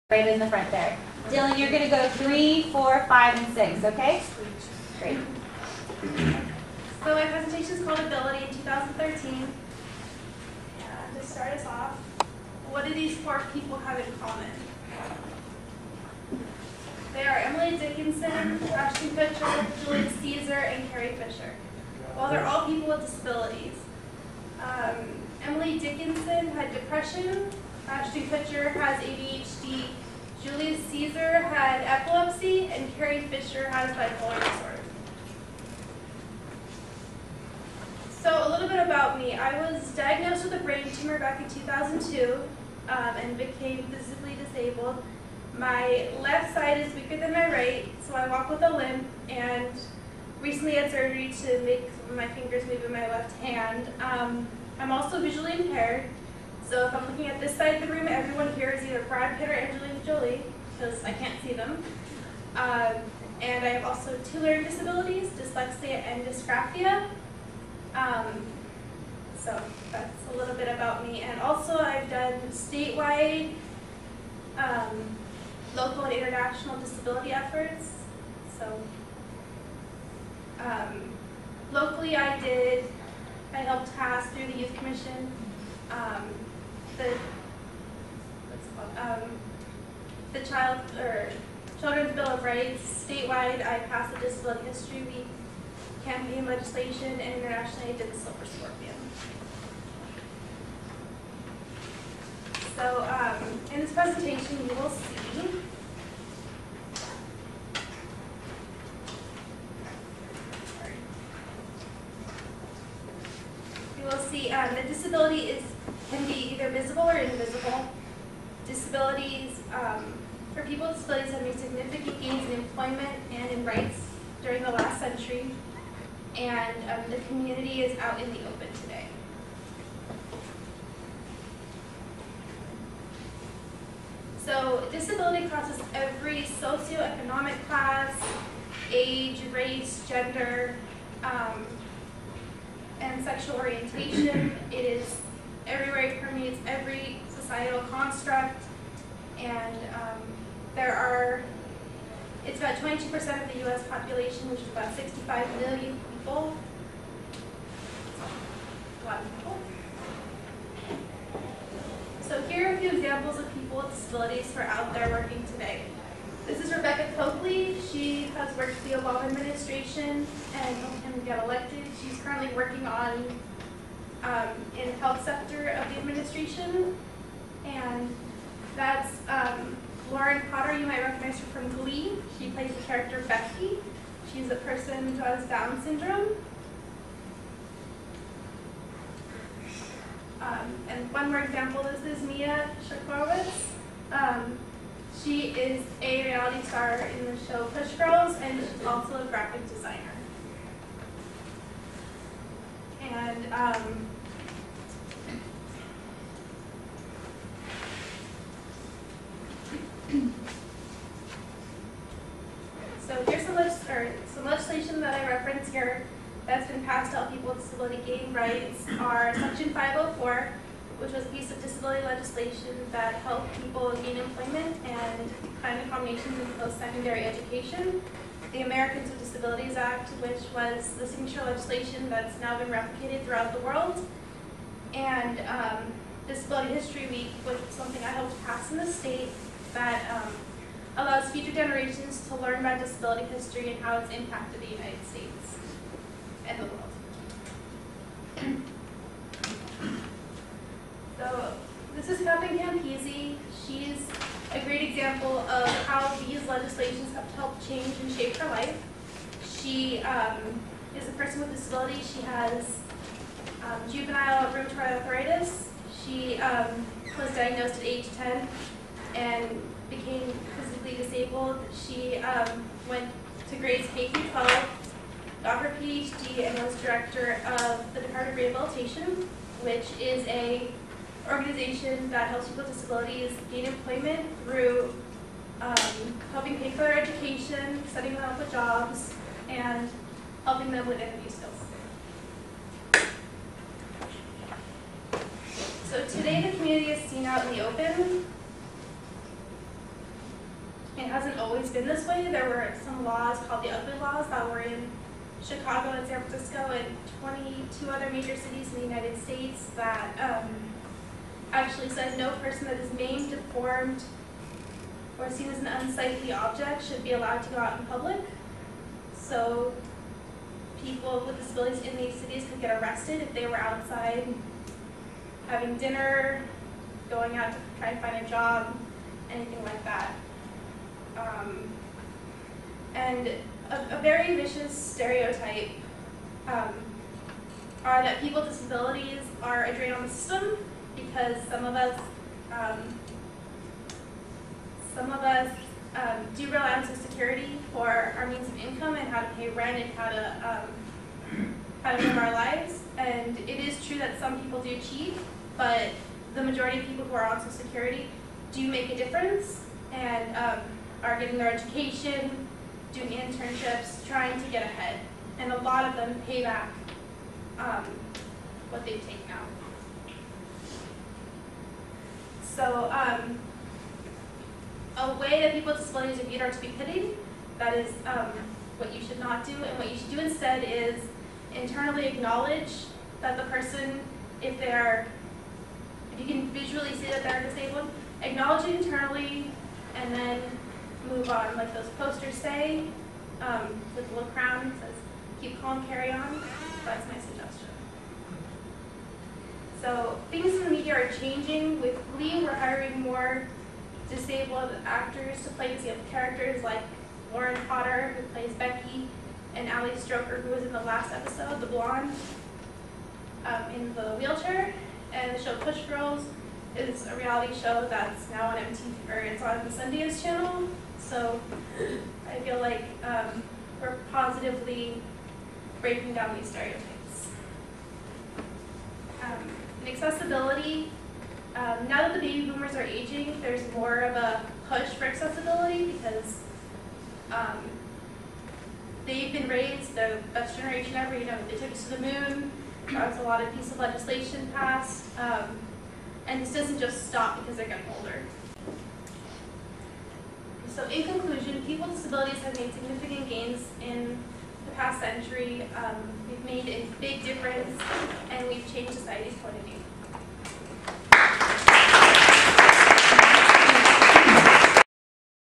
Ability in 2013
This is a speech I gave about the disability community. Topics in the speech range from accessibility,changes in media,invisible & visible disabilities, laws and employment, the community is out in the open, and a little bit of disability history